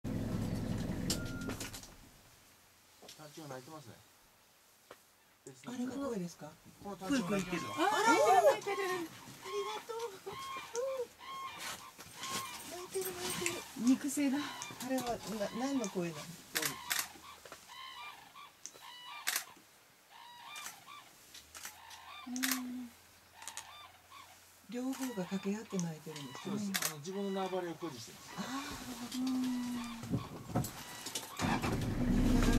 【新婚旅行アーカイブ】タンチョウ
2010年2月26日撮影　釧路湿原のタンチョウ。この時まで、タンチョウって、渡り鳥だとばかり思っていました。しかも、鴛鴦と違って、夫婦仲最高に良いらしいですね。